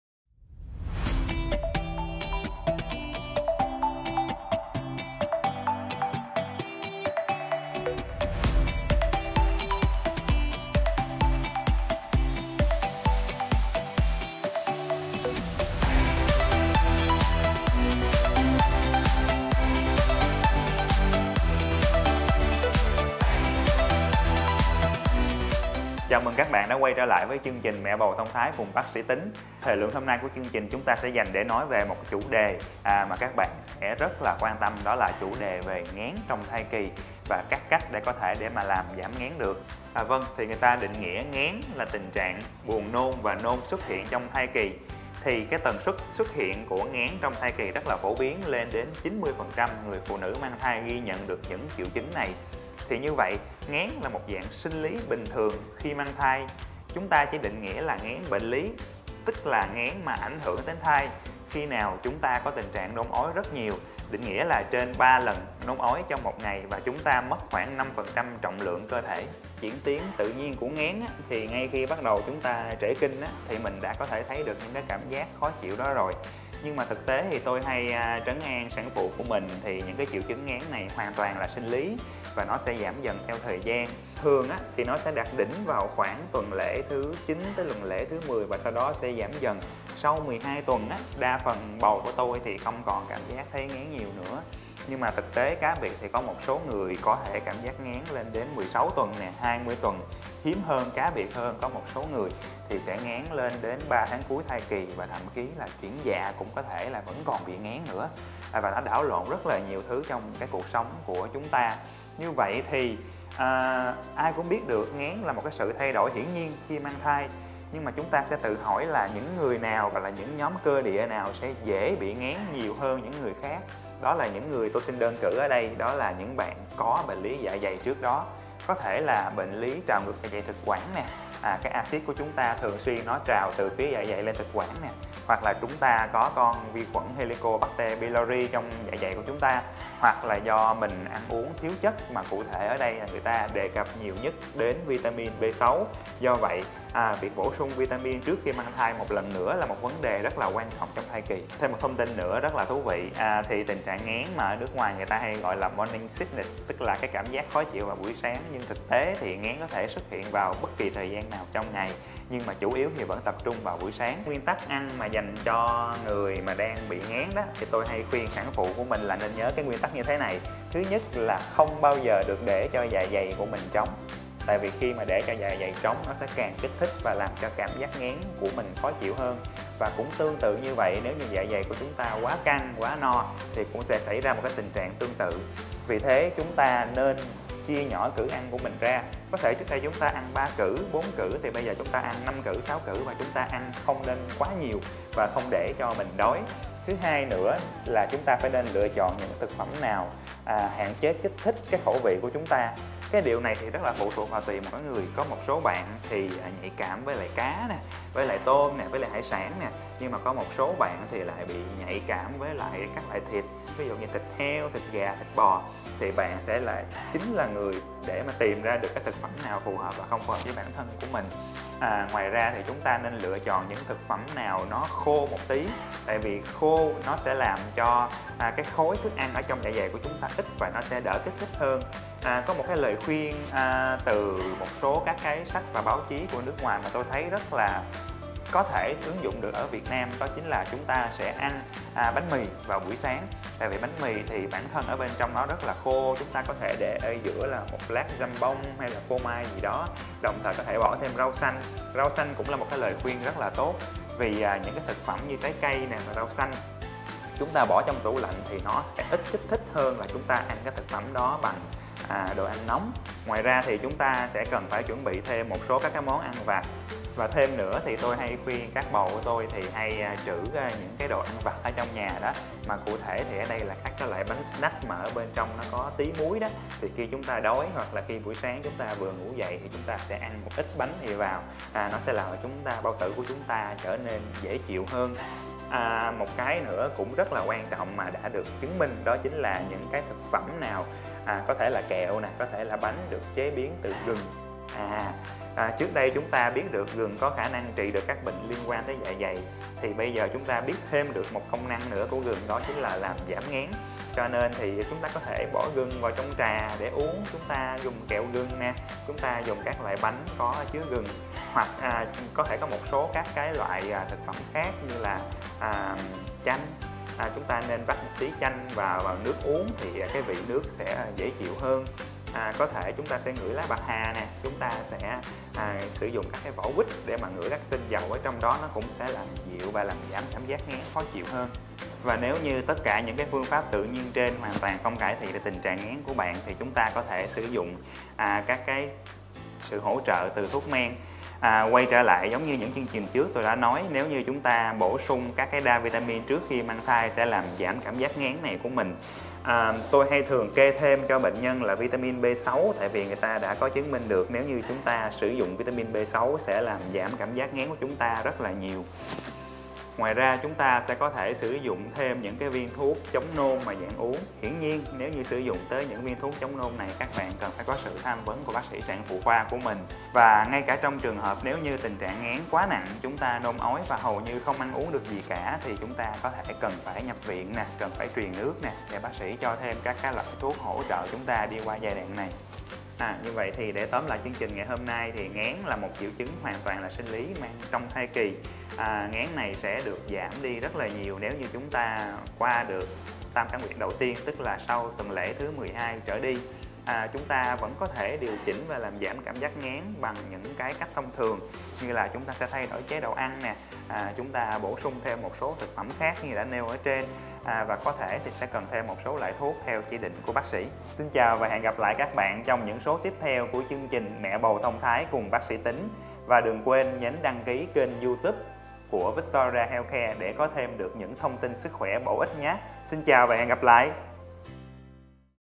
NGHÉN TRONG THAI KỲ VÀ NHỮNG ĐIỀU CẦN BIẾT
[Kiến thức Sức Khỏe Mẹ Bầu]
📌 NGHÉN TRONG THAI KỲ 📌
90% mẹ bỉm sữa hầu hết từng trải qua giai đoạn nghén trong quá trình mang thai, với những triệu chứng khó chịu, ảnh hưởng đến sinh hoạt hàng ngày.
- Vậy nghén bình thường khác với nghén bệnh lý thế nào?
- Sản phụ cần biết gì về thời điểm bắt đầu nghén?
- Với những người có bệnh lý dạ dày, nhiễm khuẩn Hp ảnh hưởng như thế nào đến tình trạng nghén?
- Chế độ ăn uống như thế nào để phù hợp với những mẹ bầu bị nghén?
Mời các bạn cùng xem giải đáp của BS. Teng Quang Tín - chuyên Sản khoa hệ thống phòng khám đa khoa Victoria Healthcare. Mong rằng chia sẻ của bác sĩ sẽ giúp các chị em có thể an tâm hơn khi bắt đầu mang thai.
👉 Tham khảo các gói chăm sóc thai kỳ tại Victoria Healthcare:
💞 HỆ THỐNG PHÒNG KHÁM ĐA KHOA VICTORIA HEALTHCARE 💞
“From Care To Caring - Chăm sóc đến từ tấm lòng"